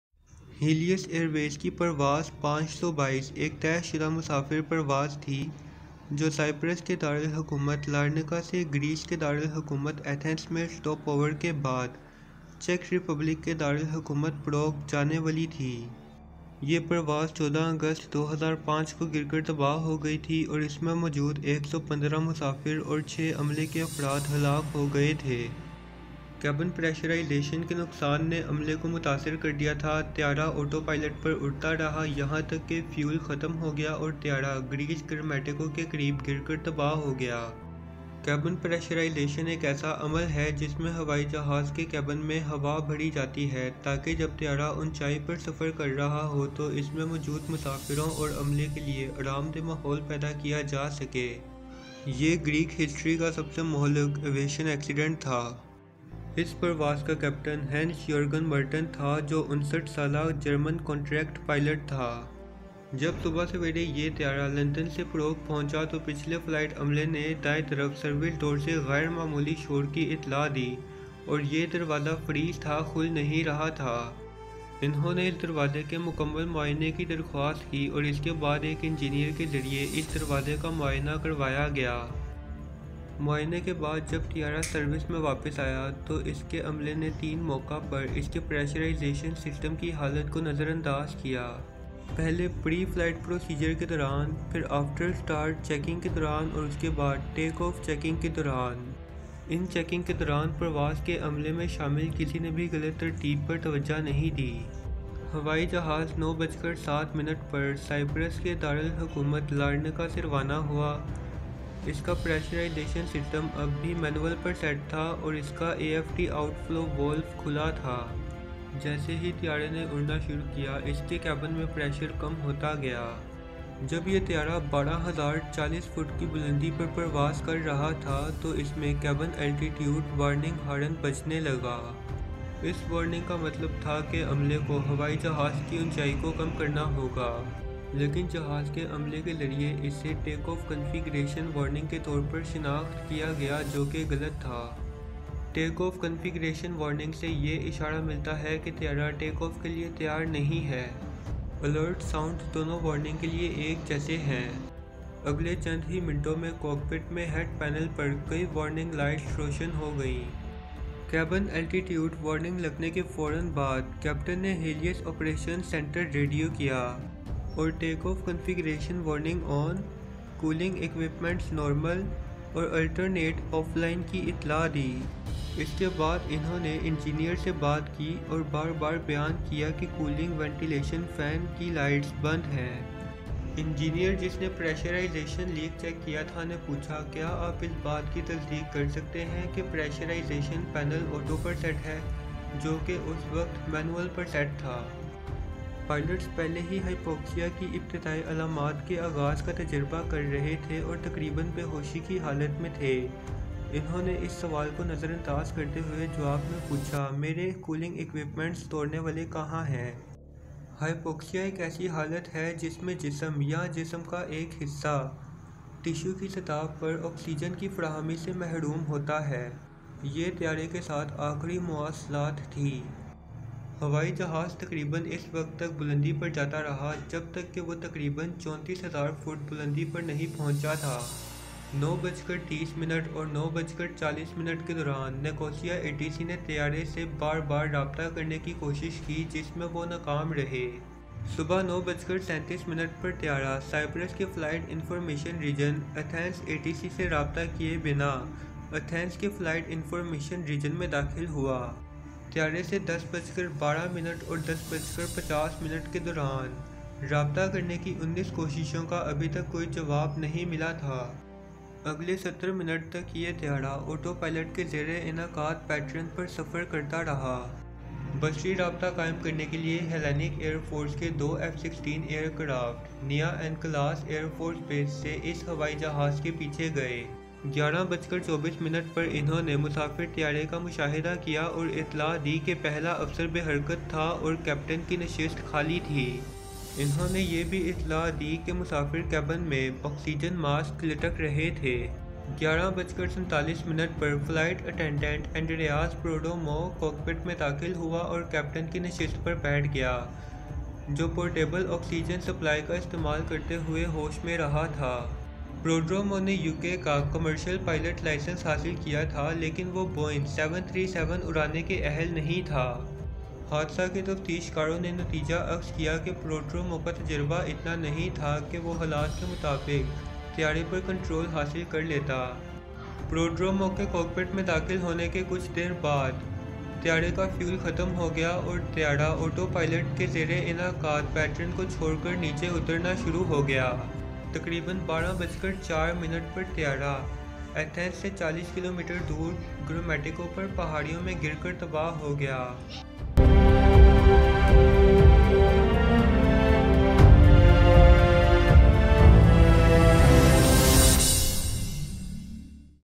Helios Airways Flight 522 that crashed on 14 August 2005
Helios Airways Flight 522 was a scheduled passenger flight from Larnaca, Cyprus to Prague, Czech Republic with a stopover to Athens, Greece, that crashed on 14 August 2005, killing all 121 passengers and crew on board. A loss of cabin pressurization incapacitated the crew, leaving the aircraft flying on autopilot until it ran out of fuel and crashed near Grammatiko, Greece. It was the deadliest aviation accident in Greek history.
Cabin pressurization is a process in which conditioned air is pumped into the cabin of an aircraft, in order to create a safe and comfortable environment for passengers and crew flying at high altitudes